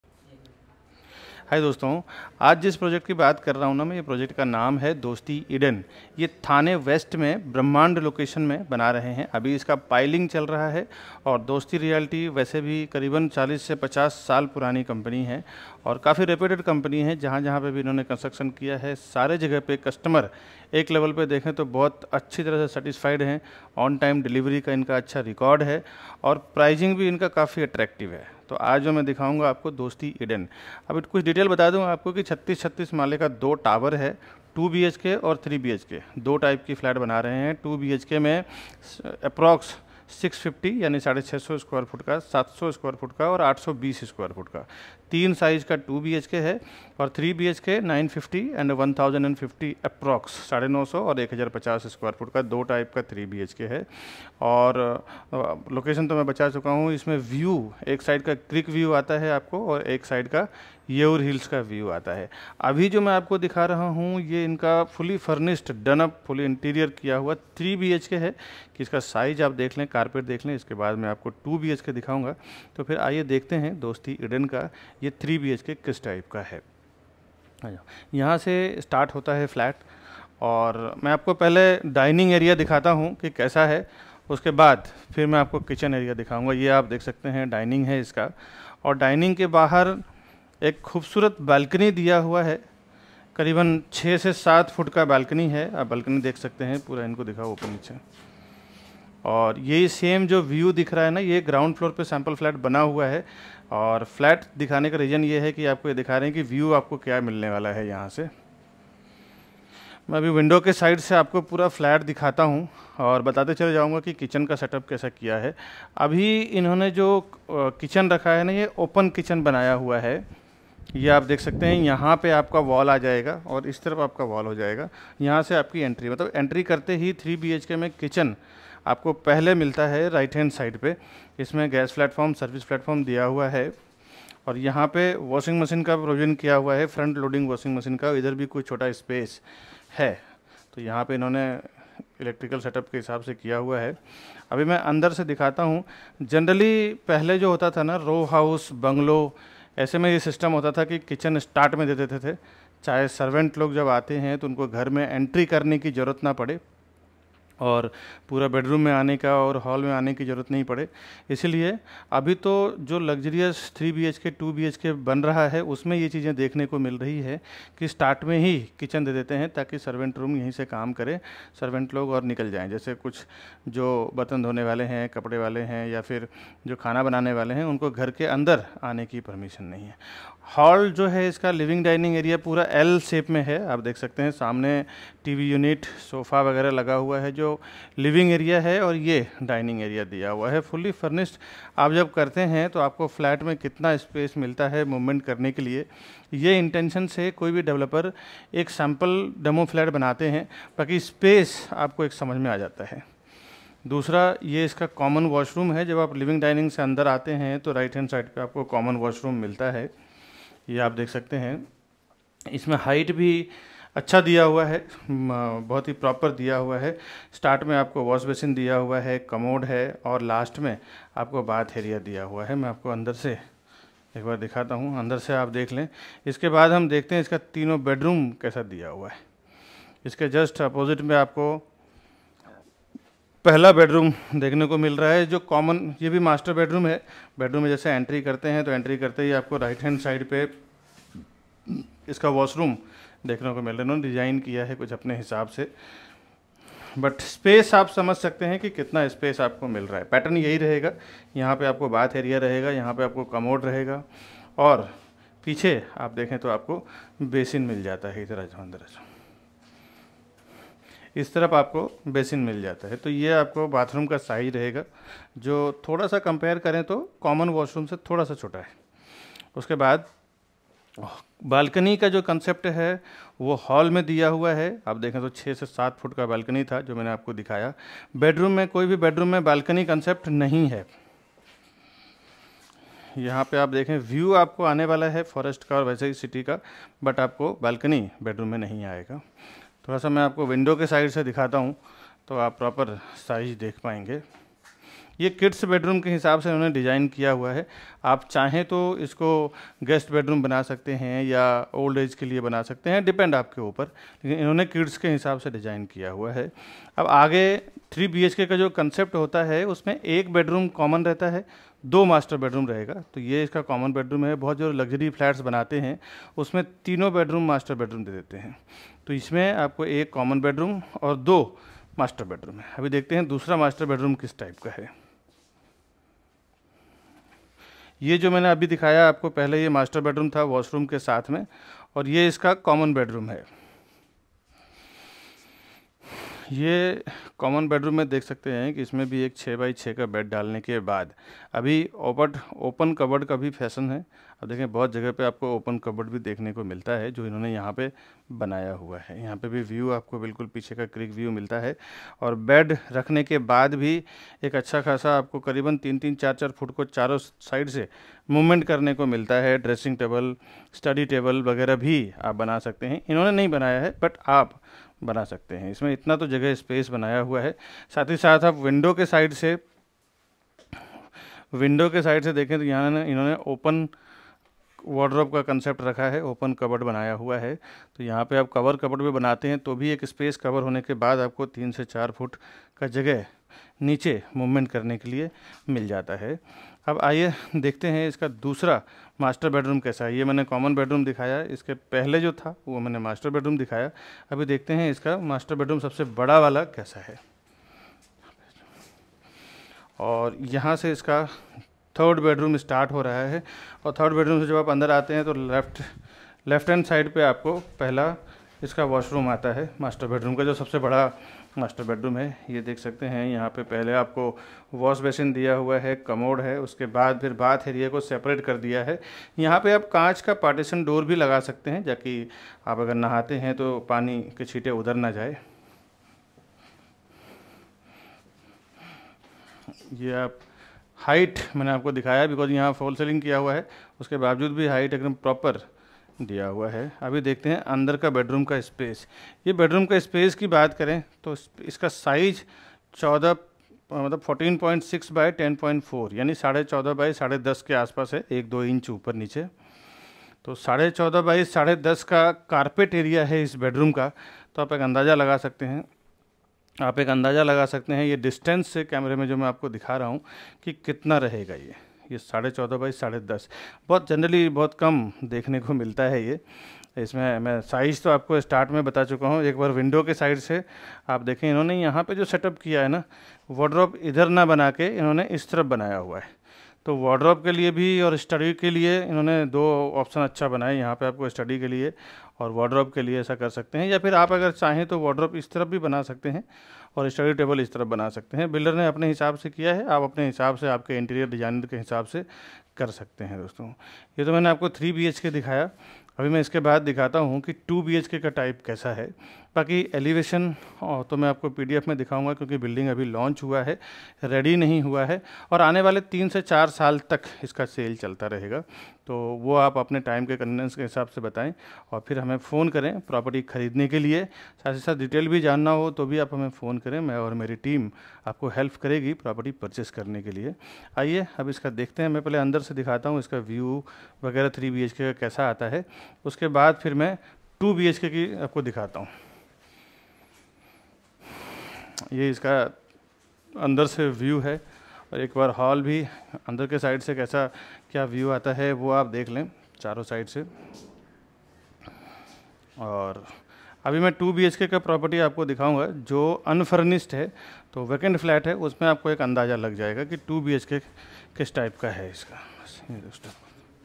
DOSTI EDEN THANE/2/3BHK/NEW LAUNCH/THANE/3BHK FLAT/PROPERTY PLAZA BIKASH BARNWAL
2BHK
CARPET AREA:- 650/702 SQFT APPROX
PRICE:- 90 LAKHS TO 1.10 CR

3BHK
CARPET AREA:- 963/1039 SQFT APPROX
PRICE:- 1.25 CR TO 1.64 CR